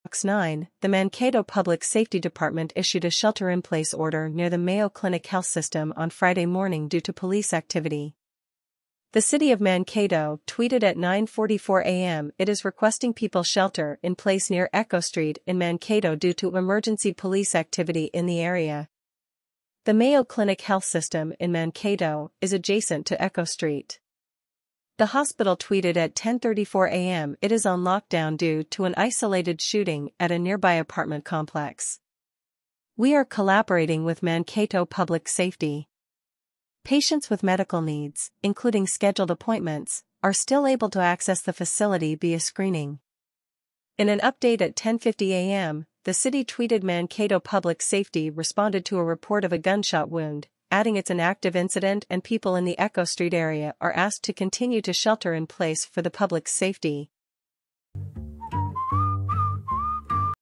Mankato Police issue shelter in place; Mayo Clinic hospital on lockdown
News Shorts and info